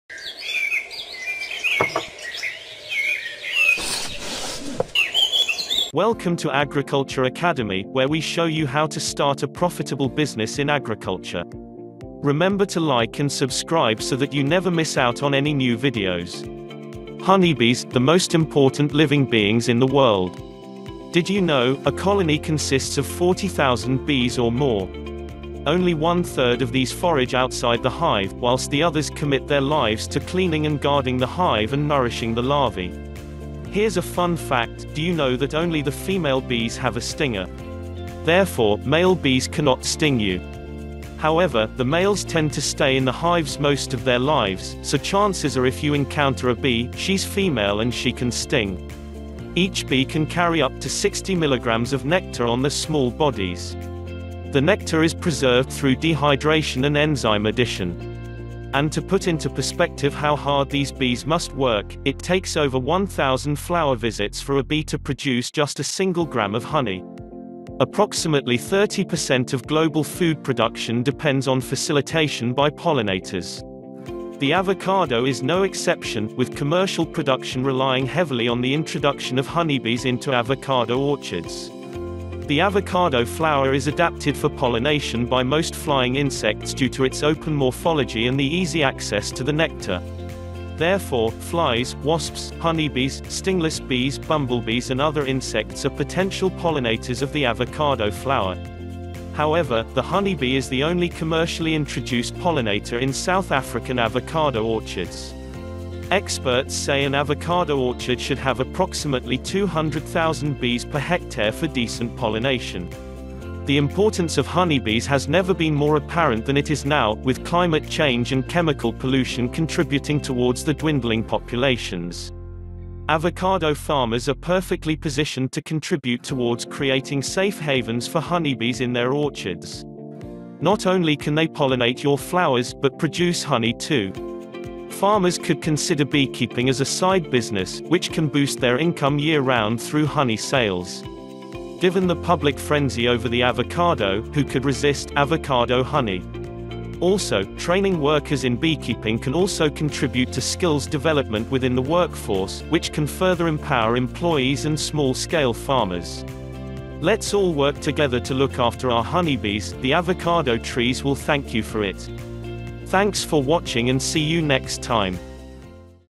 Avocado Trees Producing Nothing? Do This
Let us show you how to make avocado trees produce more fruit!

Pollinating avocados using Honeybees has the potential to increase your yield. The honeybee has recently been voted the most important living beings on earth by The Earthwatch Institute. Conserving our honeybee population has never been more important than it is now, with 30% of global food production relying on pollination.

Avocados are one of the many fruits that rely on honeybees to pollinate their flowers. Therefore, avocado farmers are perfectly positioned to contribute towards creating safe havens for honeybees in their orchards. Not only can they pollinate your flowers, but give you honey too!

🌱 Rooting hormone
🌱 Spray bottles
🌱 The Plant Propagators 'Bible'

References:
Hofshi R, Sherman G, Arpaia ML. Unknown Year. Honeybees and your avocado orchard: an argument for a bee cooperative.

This includes, but is not limited to, Camtasia, StoryBlocks, and Artlist.

Translated titles:
Polinización de flores de aguacate con abejas

Pollinisation des fleurs d'avocat à l'aide d'abeilles

Polinização de flores de abacate usando abelhas

एवोकैडो फूल को शहद के प्रयोग से प्रदूष

تلقيح أزهار الأفوكادو باستخدام نحل العسل

使用蜜蜂授粉鳄梨花

Pollinating Avocado Bulaklak Gamit ang Honeybees

Penyerbukan Bunga Alpukat Menggunakan Lebah Madu

Pendebungaan Bunga Avokado Menggunakan Lebah Madu

شہد کی مکھیوں کا استعمال کرتے ہوئے آوکاڈو پھولوں کو جرک